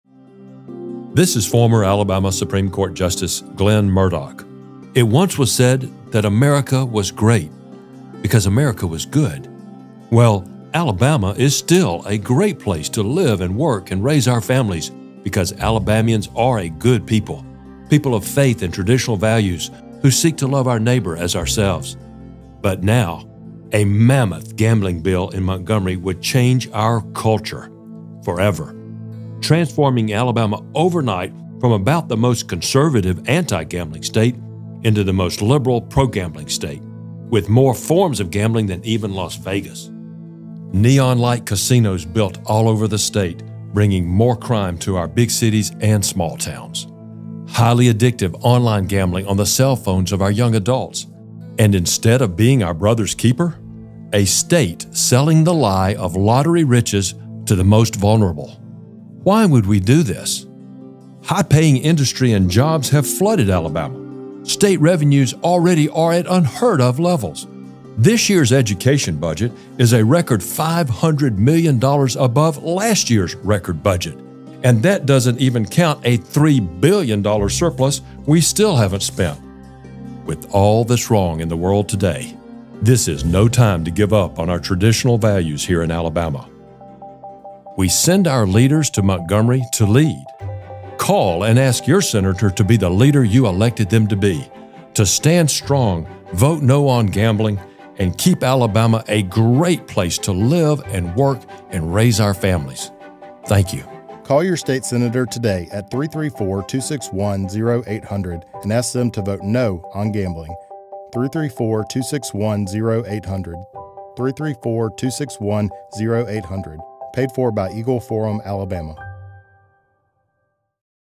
Former Justice Murdock: Gambling Hurts Alabama Families
Former AL Supreme Court Justice, Glenn Murdock expands on why gambling will hurt Alabama families.